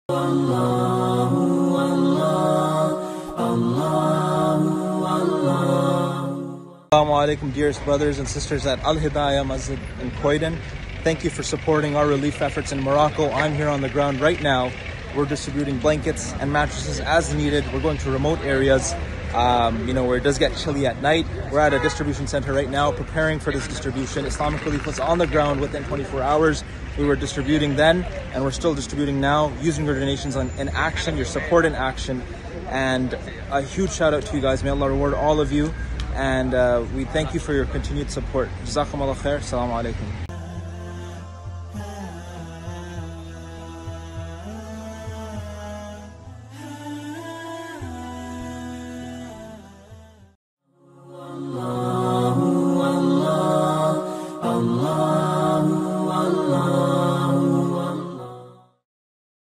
Emergency Relief Efforts in Morocco and Libya
Updates on Alhidaya Emergency Relief Efforts with Islamic Relief in Morocco and Libya...